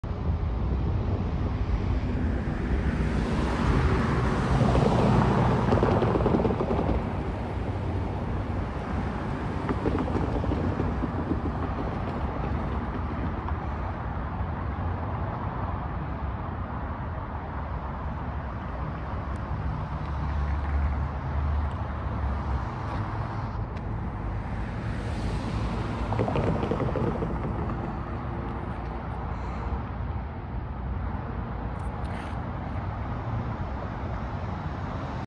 A quick look at the fire damage to the housing project off X Street in Sacramento CA!
(Filmed May 3, 2024) Here’s a quick video showing the damage from a three alarm fire at this housing project off X Street in midtown Sacramento. The resulting fire damaged the railroad crossing. It’s still unclear what exactly caused the fire. A little more info in the upcoming video for the crossing.

Twitter › chasefa...ChaseFan No. 9 (@chasefan_9) / Twitter